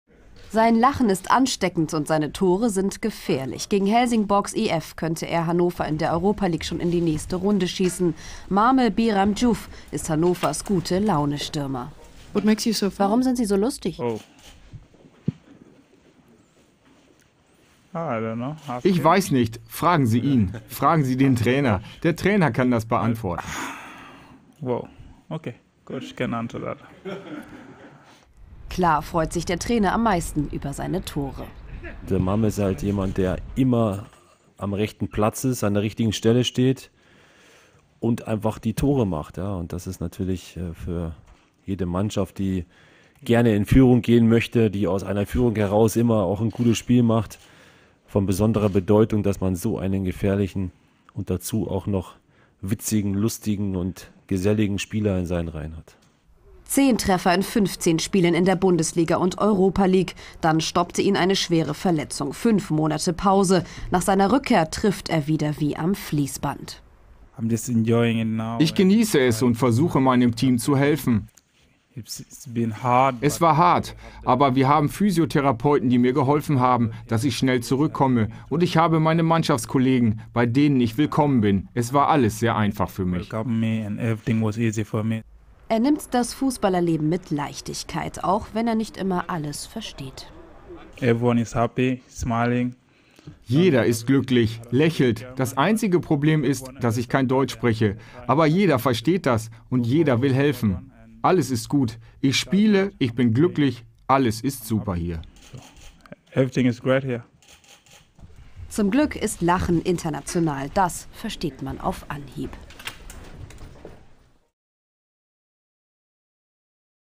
Mame Diouf trifft und trifft: Hannovers Gute-Laune-Stürmer | SPIEGEL TV
Mame Diouf trifft wie am Fließband bei Hannover 96. Auch in seiner Verletzungspause hat er seinen Humor nicht verloren. Für Mirko Slomka ist er äußerst wervoll in der Europa League und "halt einer der immer am rechten Platz ist".